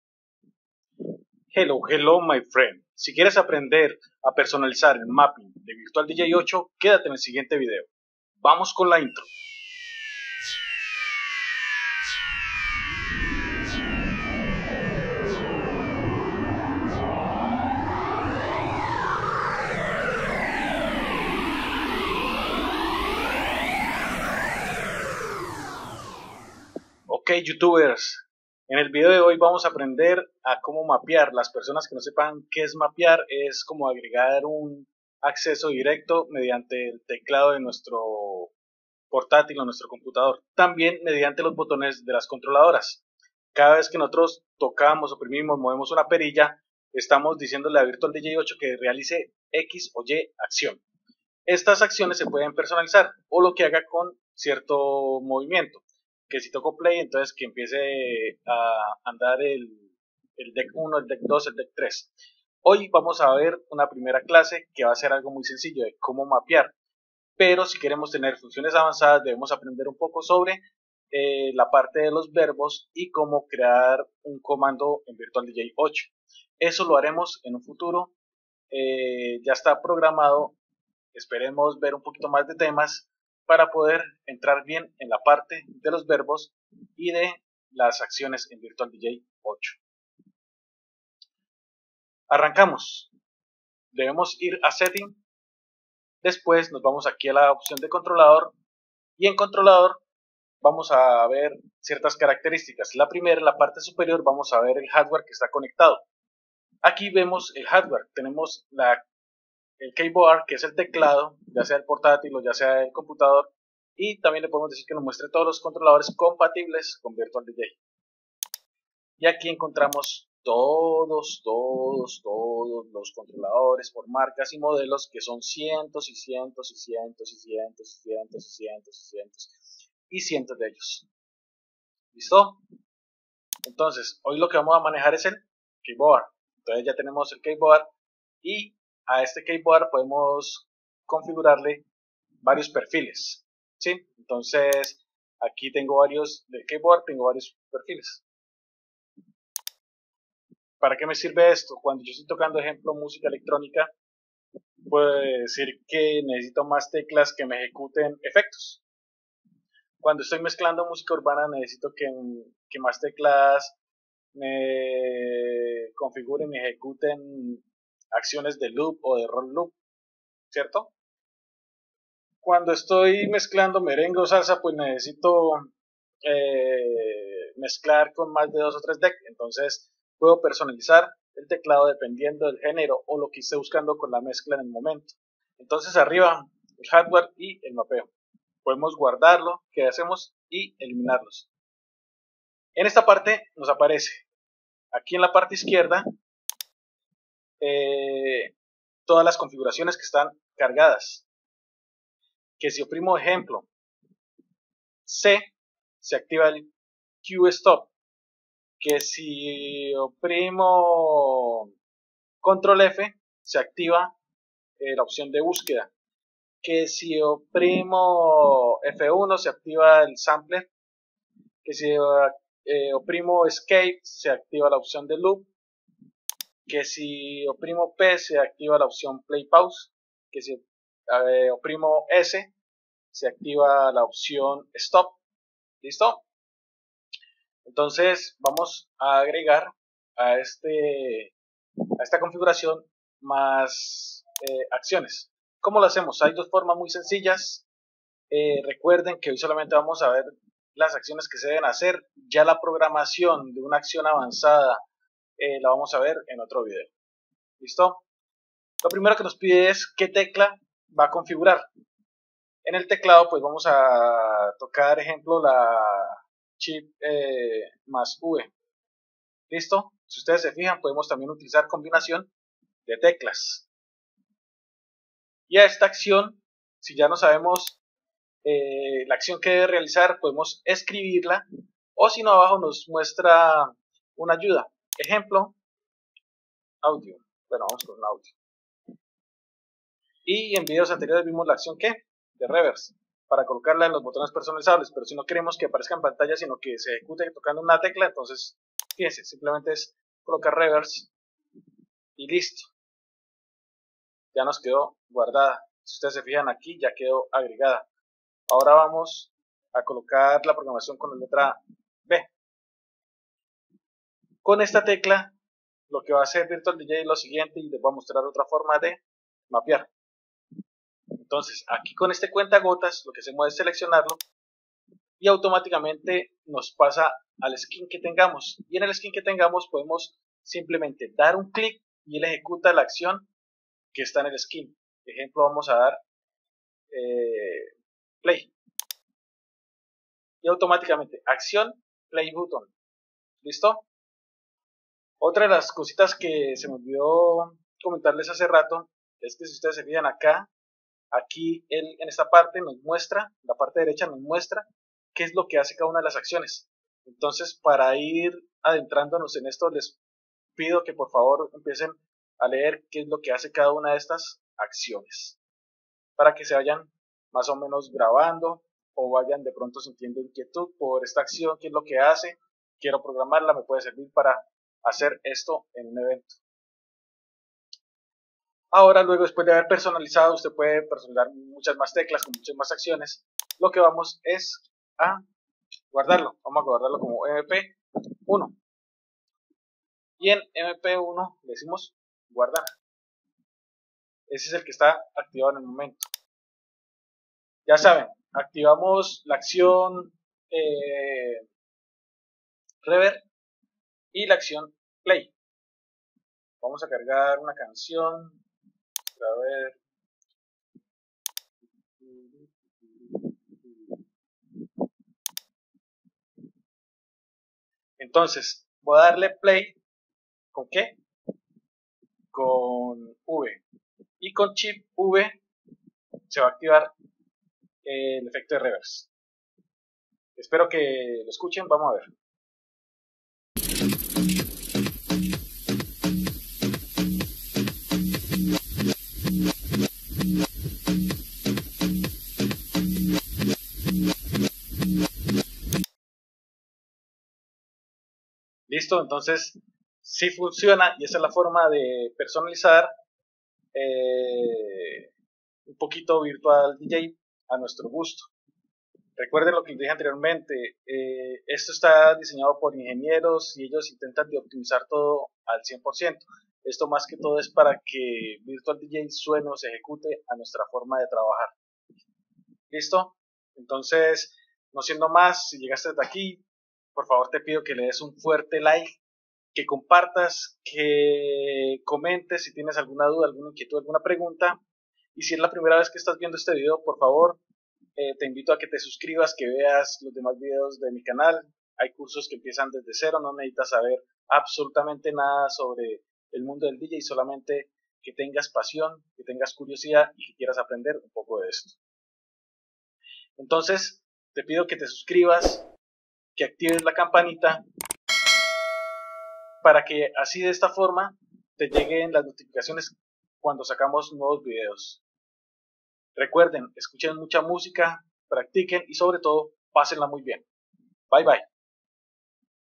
personalizar el mapping de virtual dj 8
veras los métodos para programar y personalizar el mapping en la ultima version de virtual dj